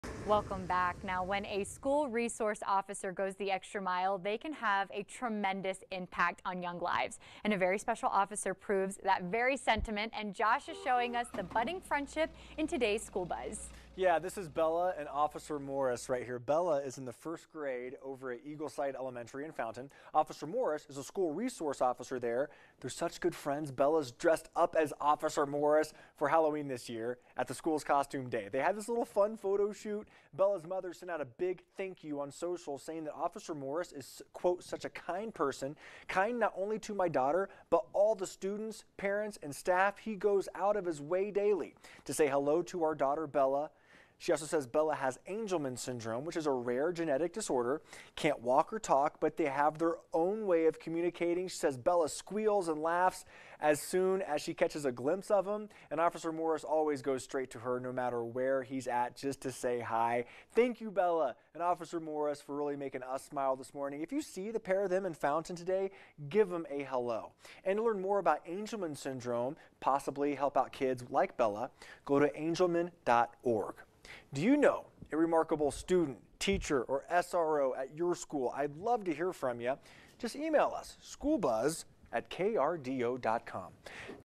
The School Buzz: Special bond forms between officer and 1st-grader at Fountain elementary ...
The School Buzz: Special bond forms between officer and 1st-grader at Fountain elementary school